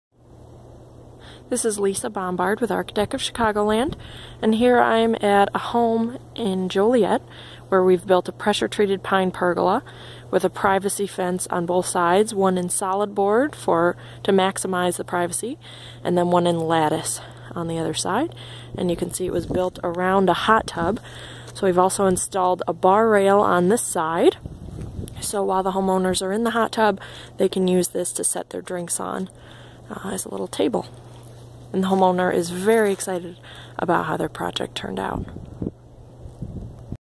Pressure Treated Pine Pergola Hot Tub Surround
This pressure treated pine pergola was built as a hot tub surround for privacy. The solid board skirting on one side features a bar rail for drinks, while the other side features lattice skirting. This pergola was custom designed and built by Joliet, IL pergola builder, Archadeck of Chicagoland.